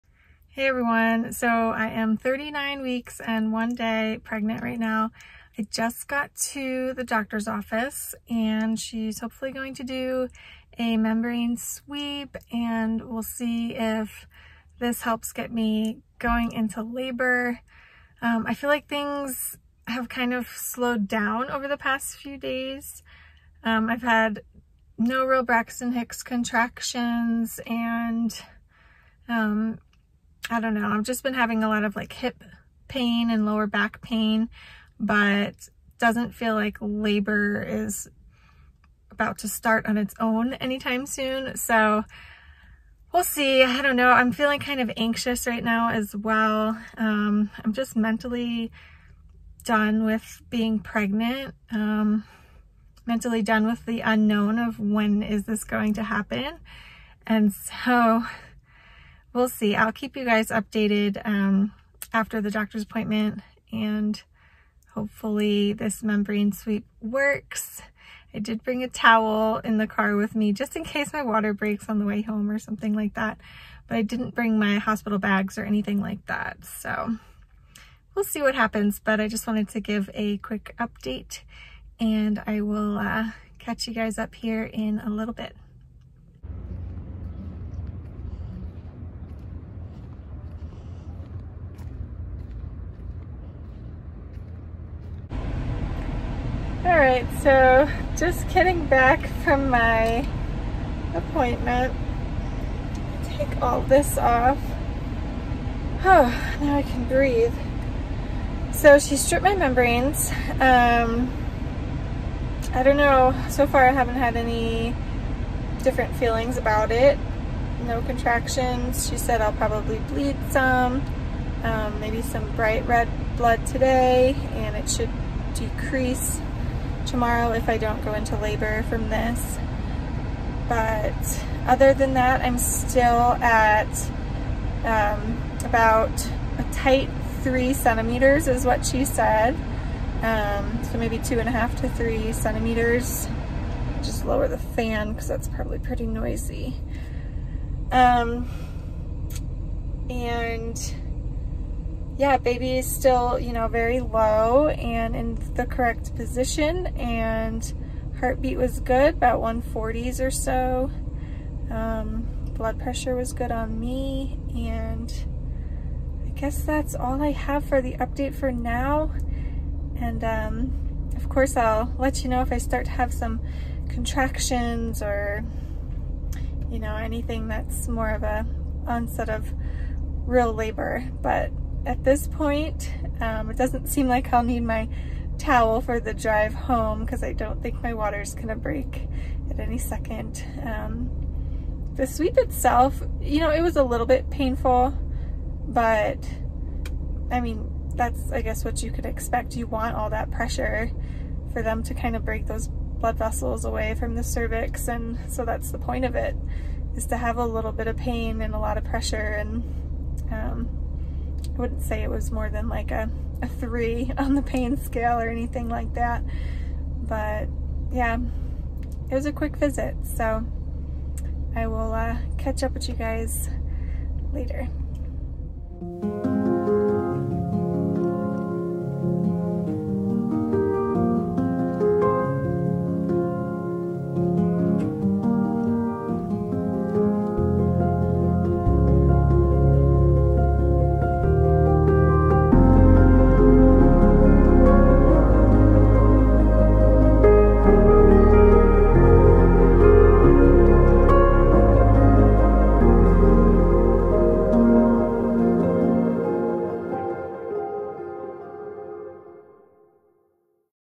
QUICK PREGNANCY UPDATE AT 39 WEEKS | DID THE MEMBRANE SWEEP WORK?
I’m about to have a membrane sweep done at 39 weeks pregnant! Here’s my experience!